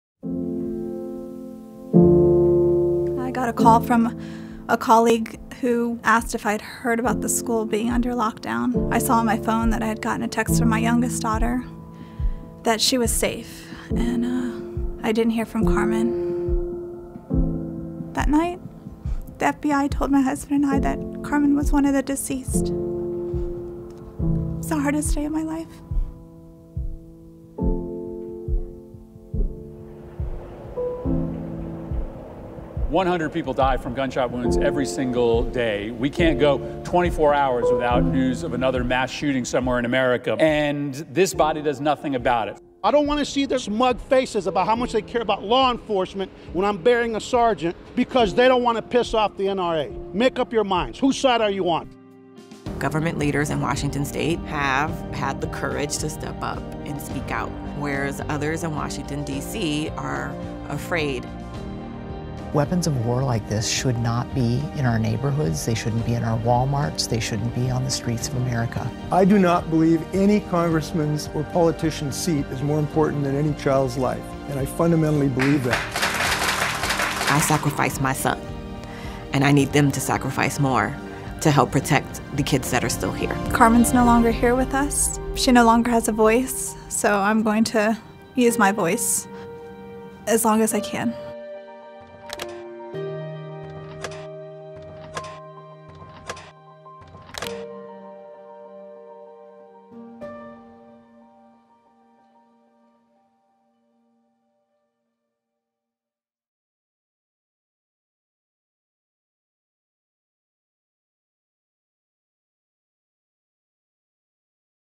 A Tale of Two Washingtons
Before COVID19, the theme of our 8th annual luncheon fundraiser was set to be A Tale of Two Washingtons. Like everything else, our luncheon is looking different this year, but this story is the same. In the face of federal inaction, Washington has led the fight against gun violence.